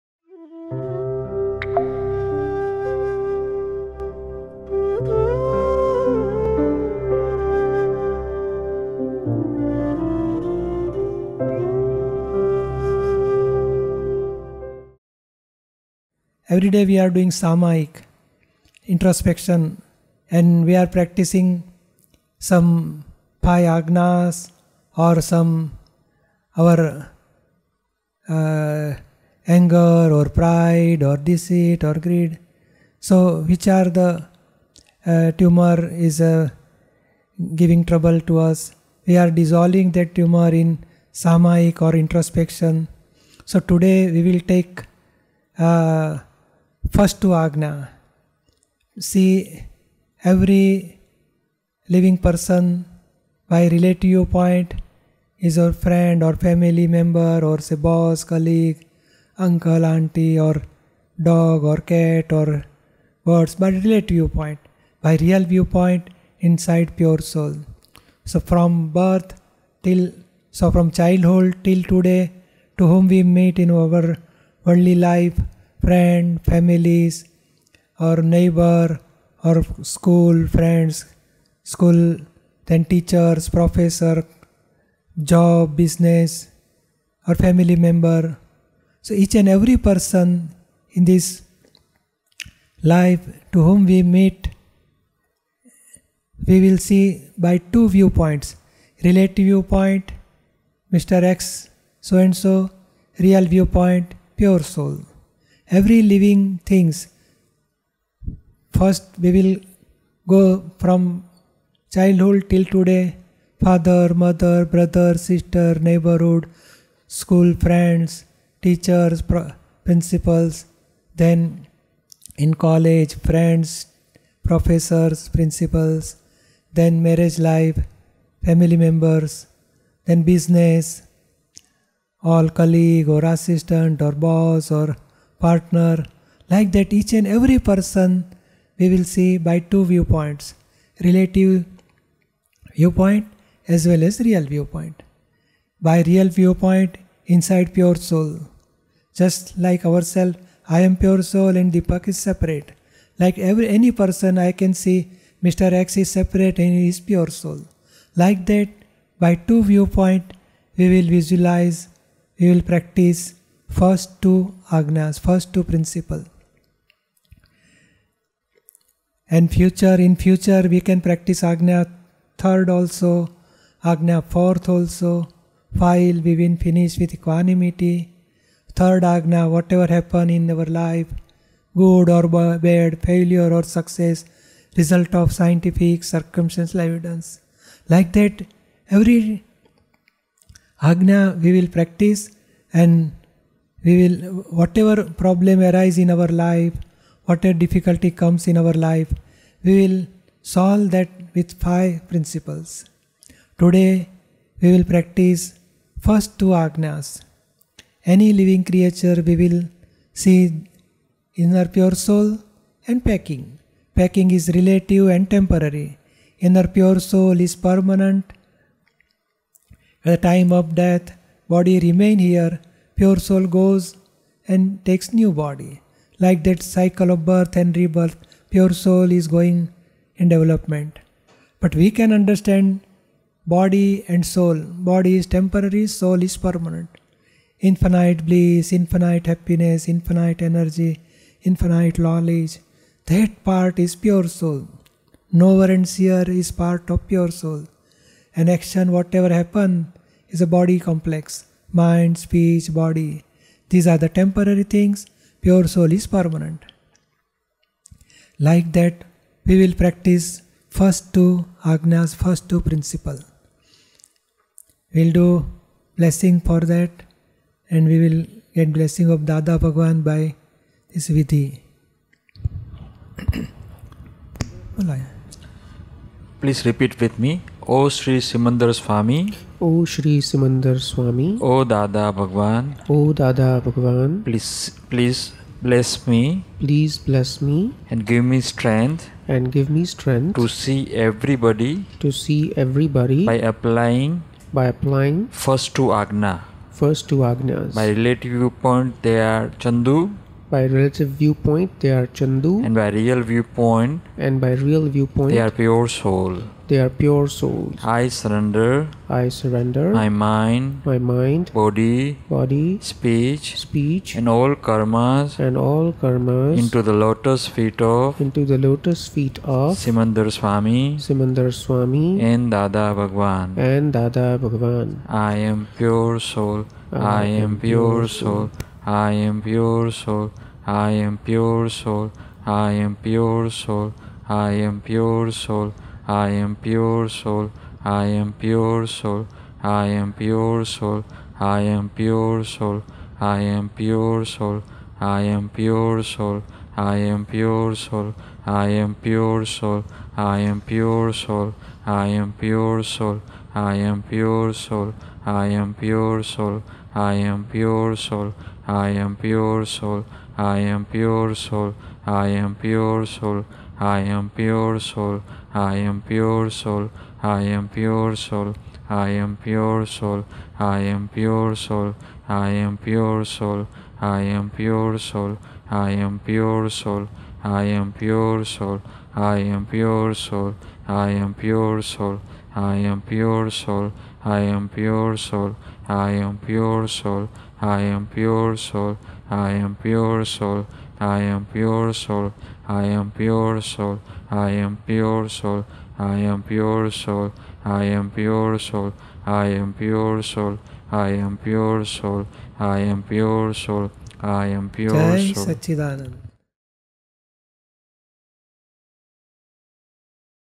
Practice of Seeing Everybody with Real and Relative View Point | Pujyashree Deepakbhai
In the present video, Pujyashree Deepakbhai explains the Samayik (introspective analysis as the Self) on how to practice seeing everybody with real and relative view point.